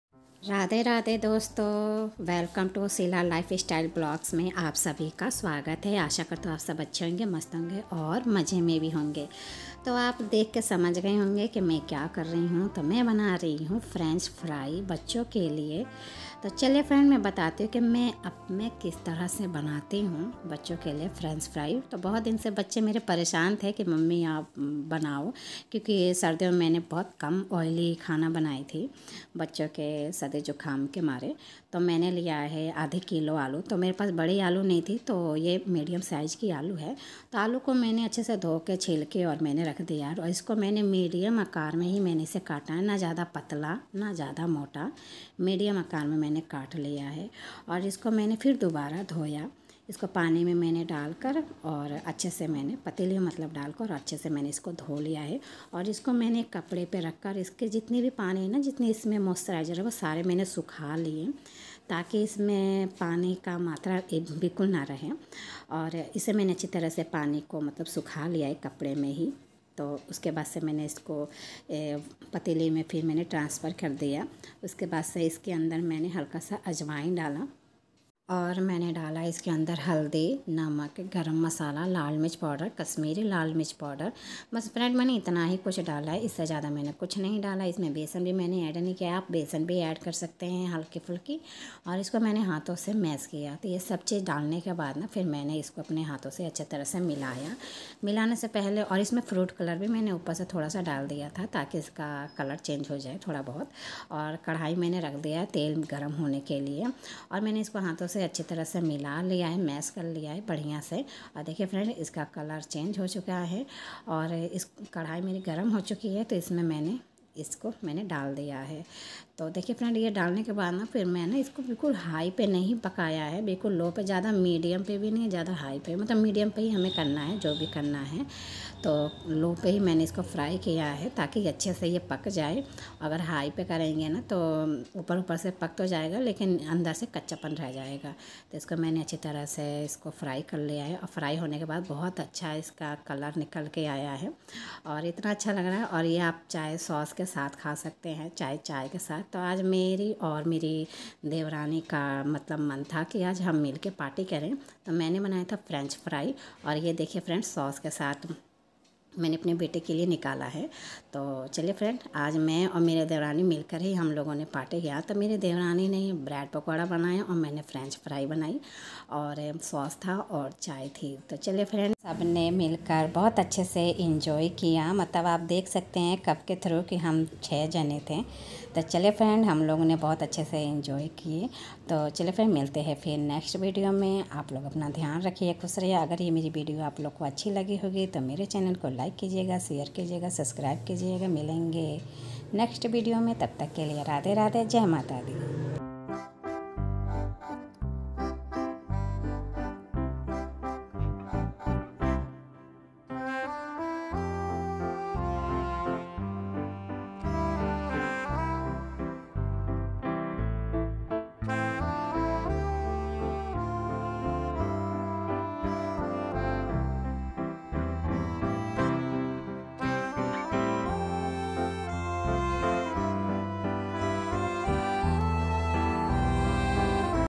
🌺 बहुत दिनों के बाद आज मैं मेरी देवरानी मिलकर बच्चों के साथ पार्टी की
राधे राधे बहुत दिनों के बाद आज मैं और मेरी देवरानी मिलकर बच्चों के साथ पार्टी की जिसमें मैंने आज फ्रेंच फ्राई बनाए आलू के अगर यह वीडियो आप लोग को अच्छी लगी होगी तो मेरे चैनल को लाइक शेयर सब्सक्राइब कीजिएगा राधे राधे

# फ्रेंचफ्राईआलूके  # हमनेमिलकपार्टीकी  # आलूफ्रेंच